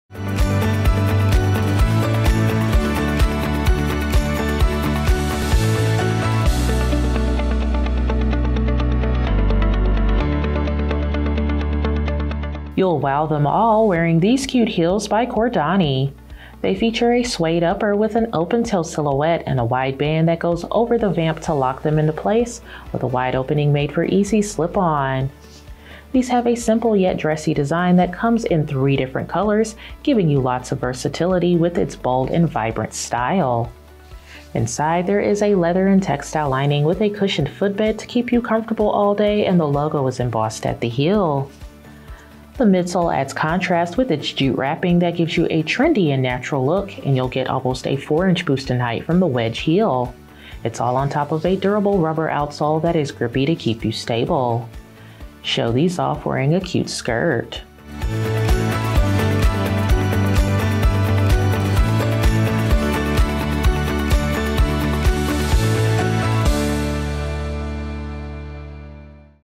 Cordani Easton SKU: 9810256
Experience comfort all day long wearing the stunning and super comfy Cordani&trade; Easton heels.
Man-made upper and lining.
Lightly padded leather footbed.
Slide-in style.
Open toe and jute-wrapped wedge platform heel.
Rubber outsole with high traction and durability.
Made in Spain.

Product measurements were taken using size EU 40 (US Women's 9), width M. Please note that measurements may vary by size.